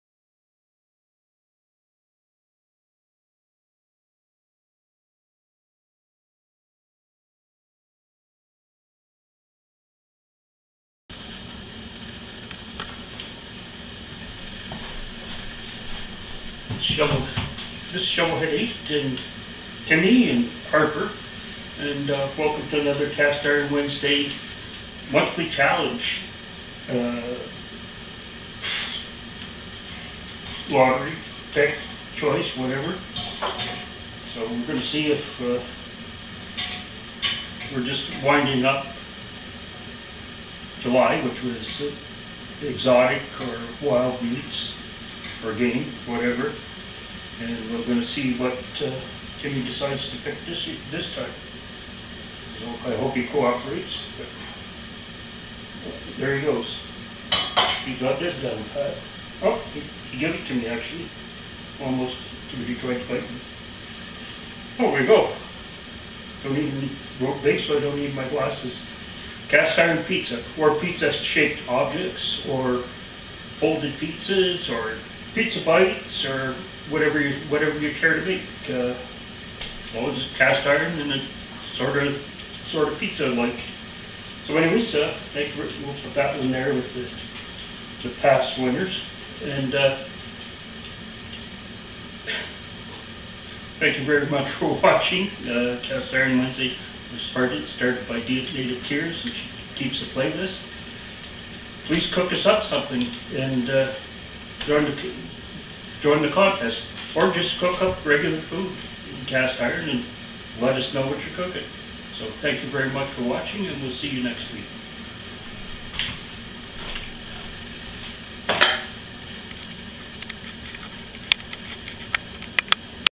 Cast Iron Wednesday Monthly Challenge for August! And the Winner is
New challenges added to the pot as we get them
For the Play list go see Dee at Native Tearz Come and join us for another great challenge! or a regular favorite of yours. Get out your Cast Iron and Get Cooking!
Add your suggestions and we will put them in the pot